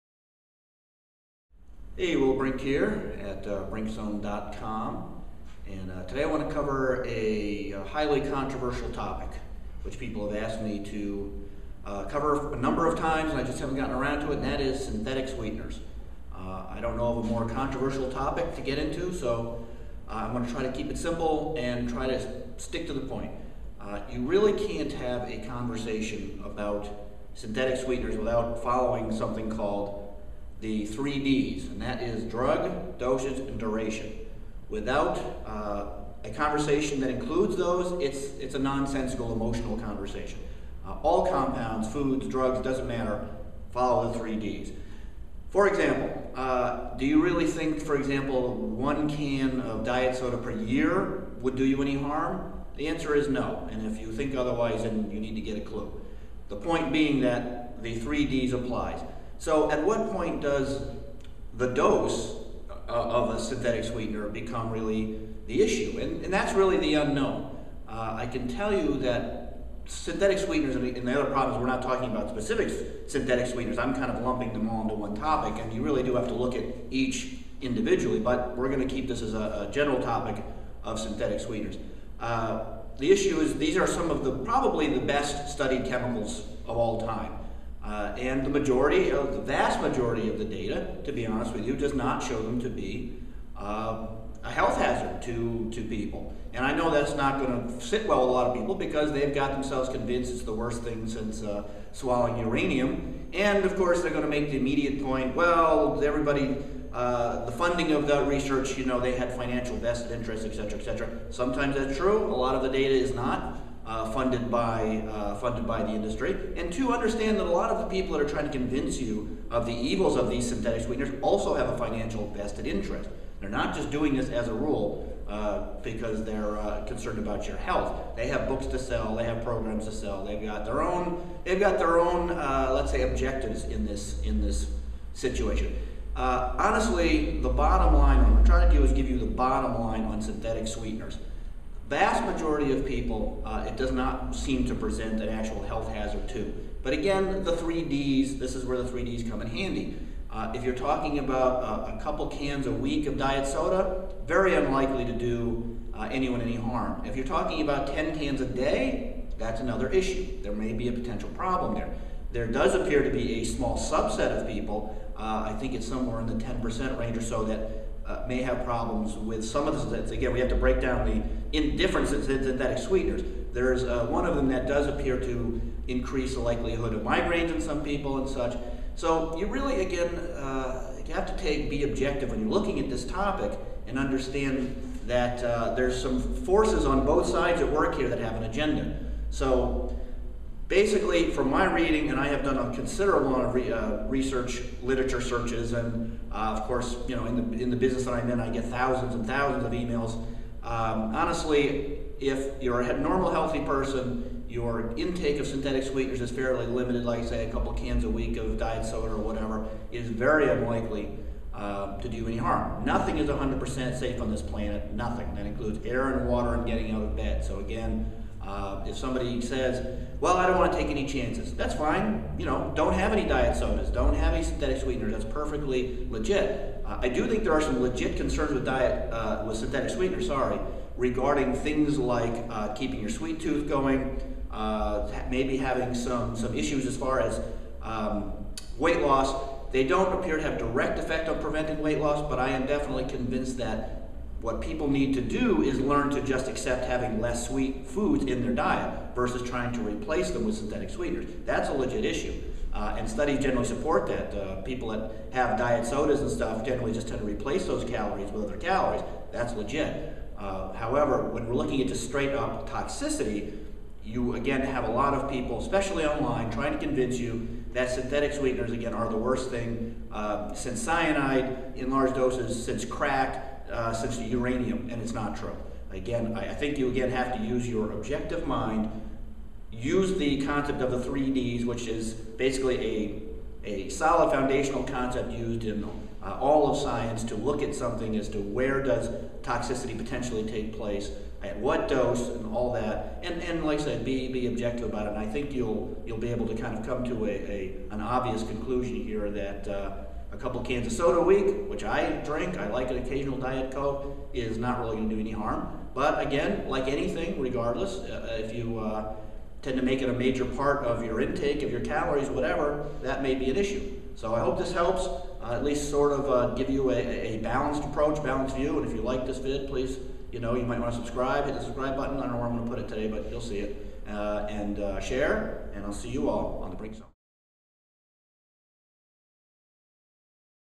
Artificial Sweeteners: THE FACTS
Synthetic sweeteners, toxic or safe? In this vid, I get to the bottom of this debate!